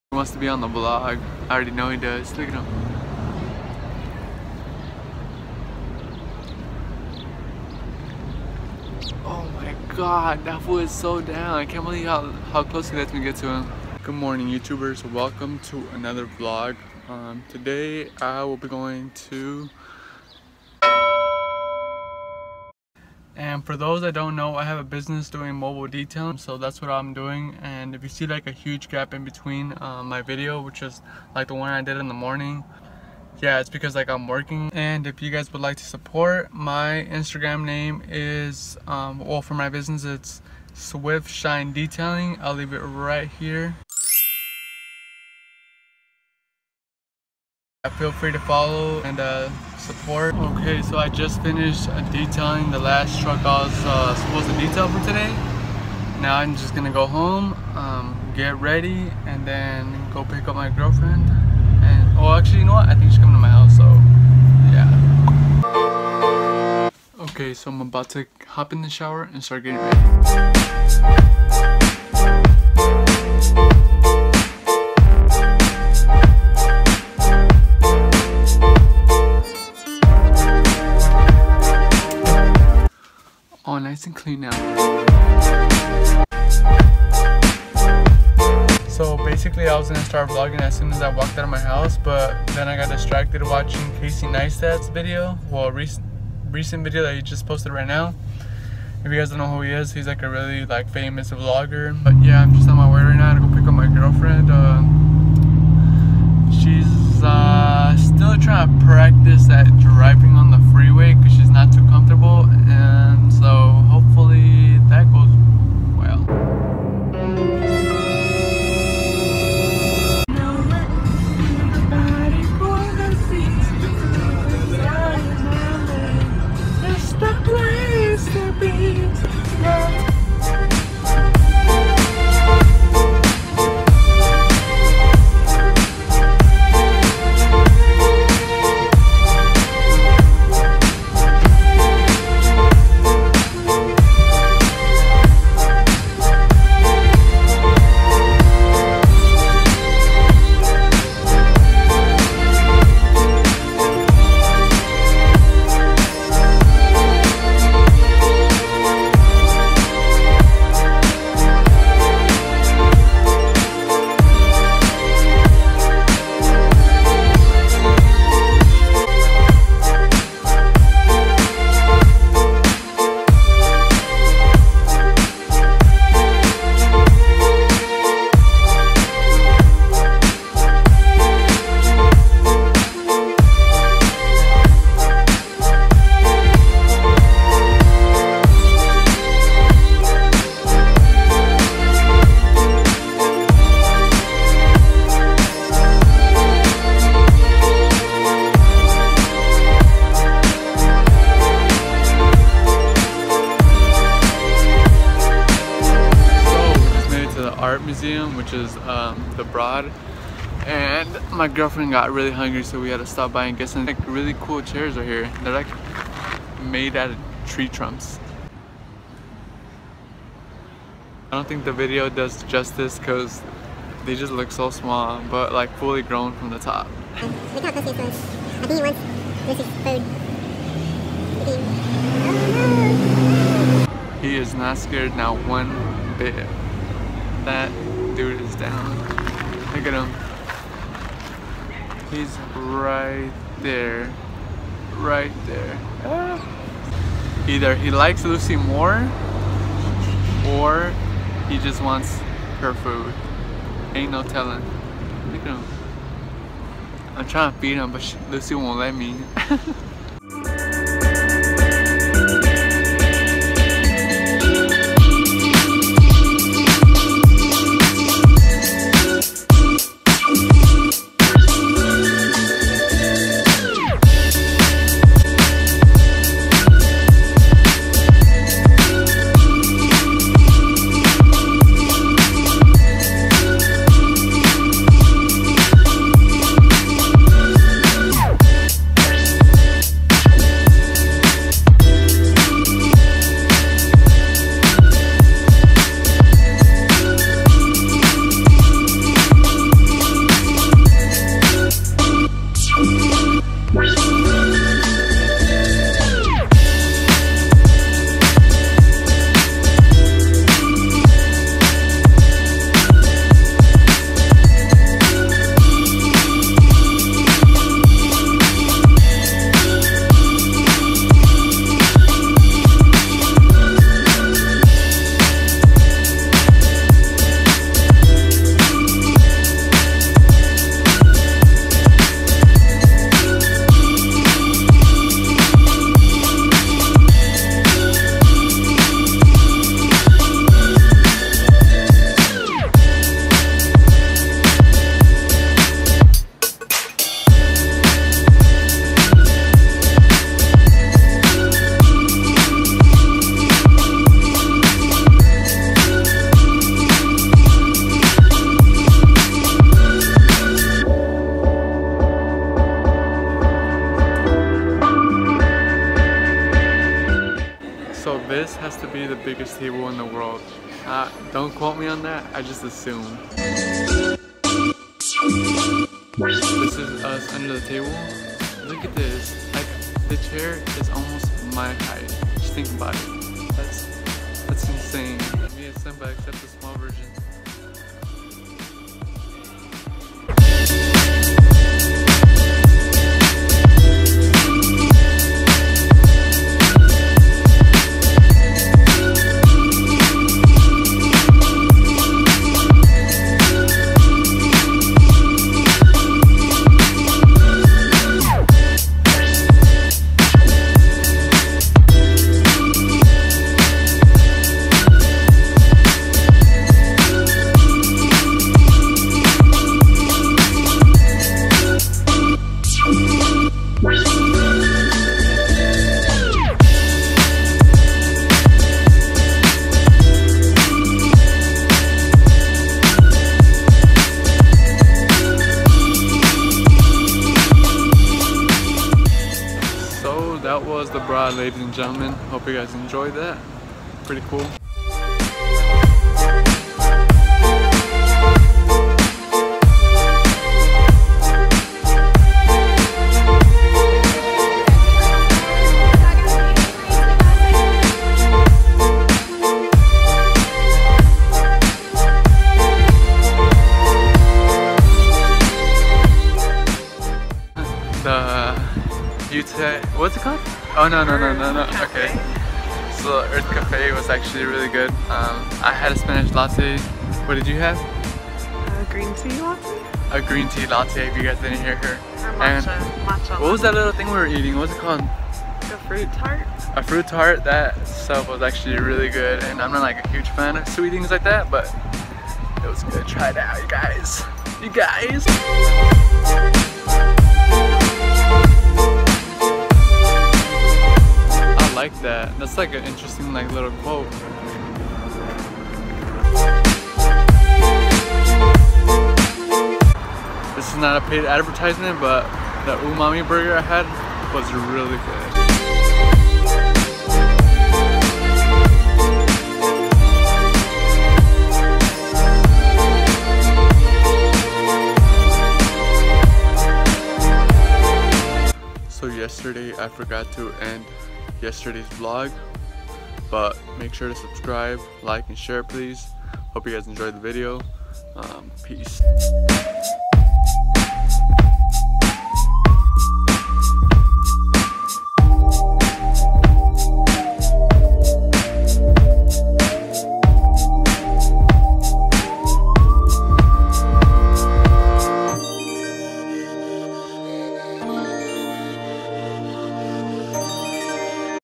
l SHOCKING l Table bigger than humans (The Broad Museum)
Heres a video of the art museum in los angeles "The Broad. Hope you guys enjoy and make sure to always show some love. leave some feedback on comment box below.